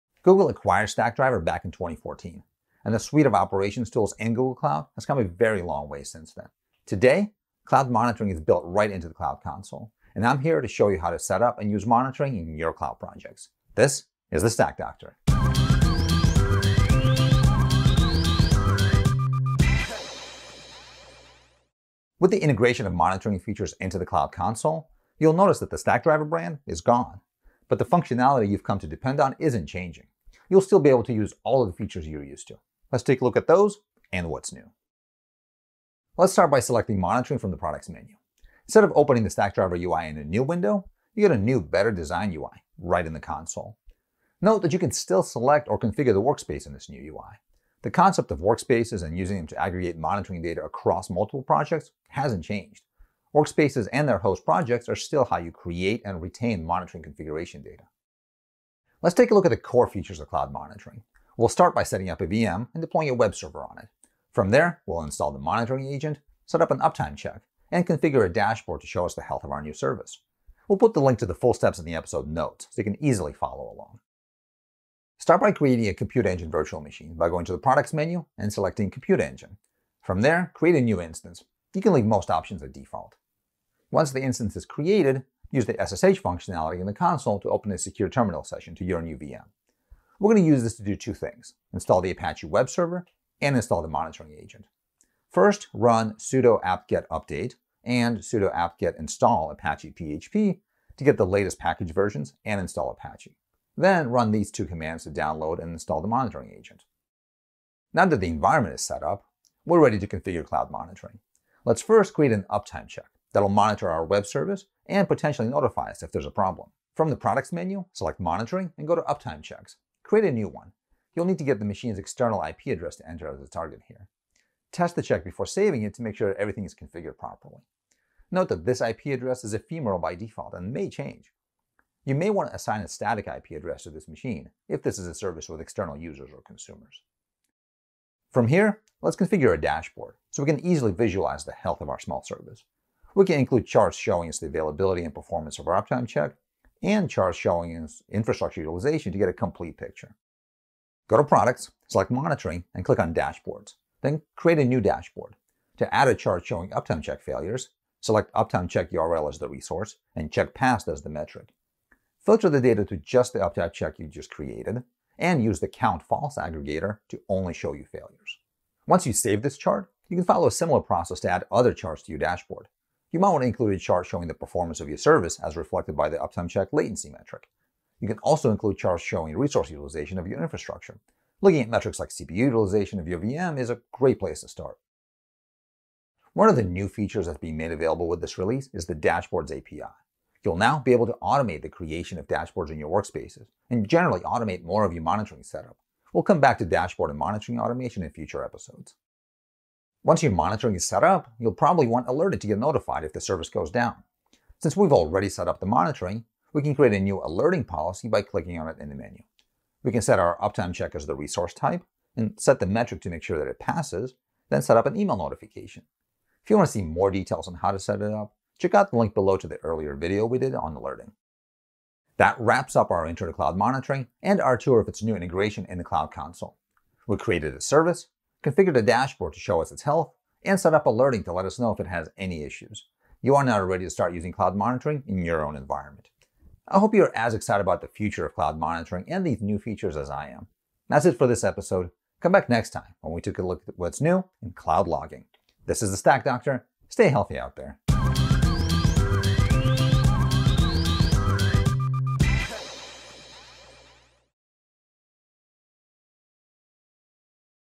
Getting started with Cloud Monitoring
Want to know how to utilize Cloud Monitoring in Cloud Console? In this episode of Stack Doctor, Yuri Grinshteyn shows you how to set up a simple web service and instrument it for both service health and infrastructure monitoring.  Watch to learn how you can navigate Cloud Monitoring’s new integration into Cloud Console and gain better observability of your applications and infrastructure.

Product: Google Cloud Operation Suite, Cloud Console, Cloud Monitoring; fullname: Yuri Grinshteyn;